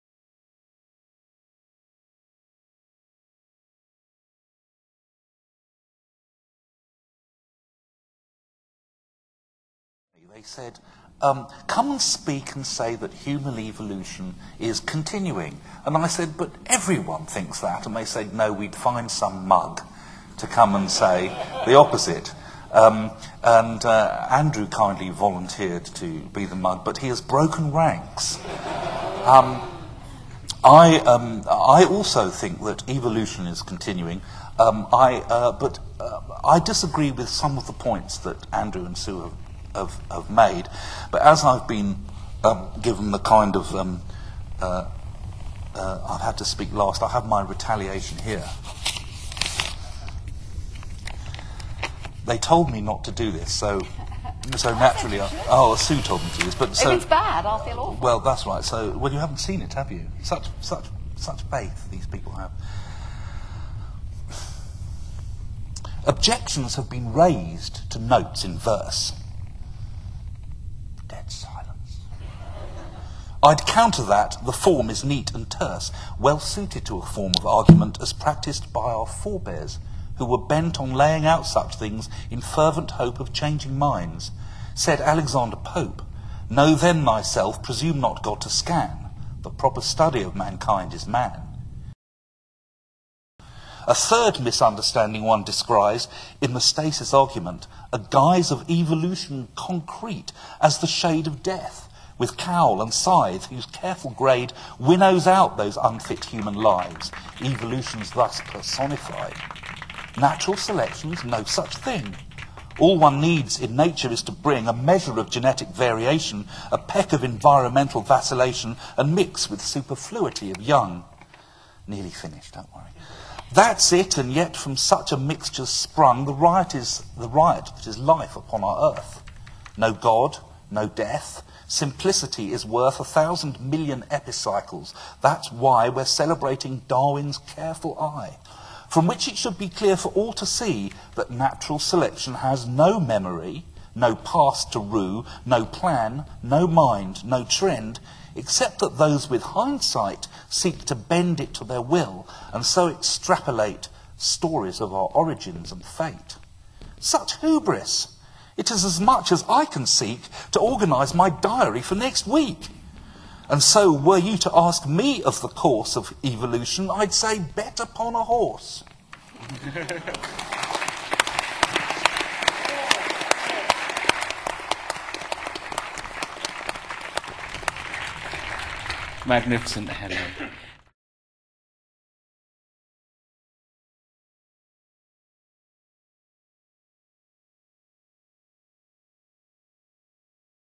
Nature Debate: Are We Still Evolving (snippet)
Held by the Nature magazine folks at King's Hall in London and in Second Life, this was a wonderful chat. I have permission to share a snippet I frapped. Enjoy select portions of Henry Gee's response in verse!